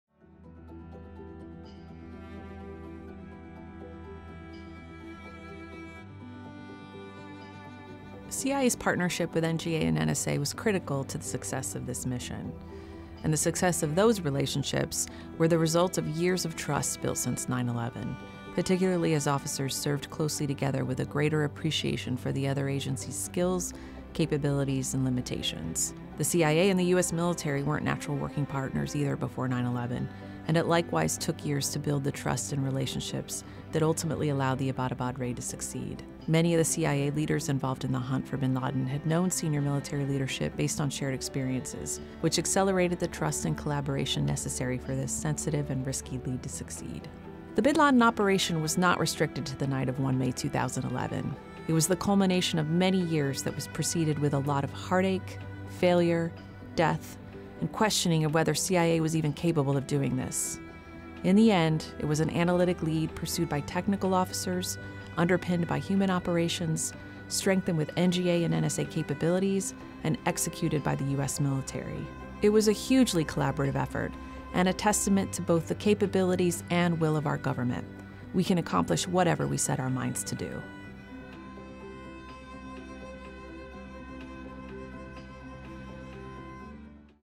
The Debrief: Behind the Mission - The Hunt for Bin Ladin, Part 4
The mission to capture Usama Bin Ladin was the culmination of many years of complex, thorough, and highly advanced intelligence operations and analyses led by CIA and partners across the Intelligence Community (IC).